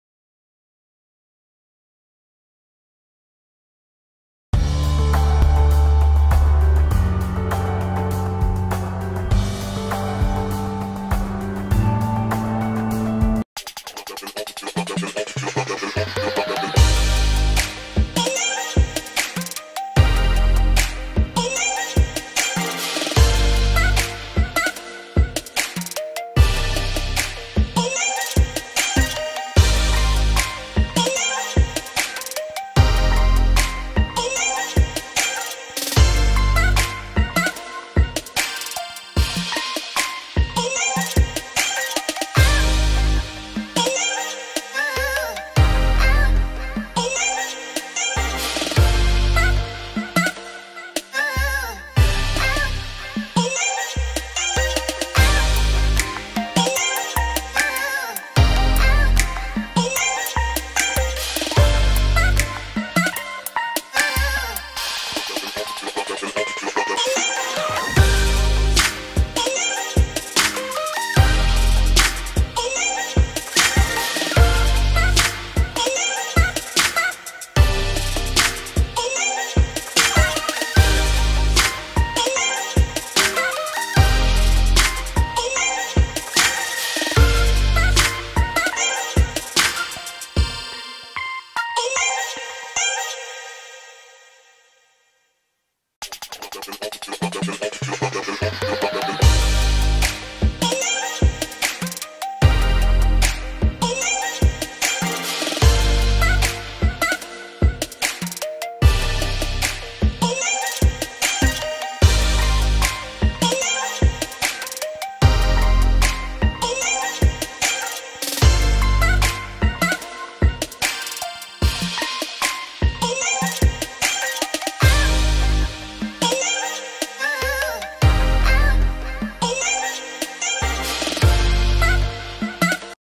MGA KANDIDATA NAGSAMPLE NG PASARELA
Here are some video clips of SAMANTHA OBDAM, RABIYA MATEO AND ANDEA MEZA showcasing there catwalk. Taken @sashfactorinternational @dyancastillejo @andreameza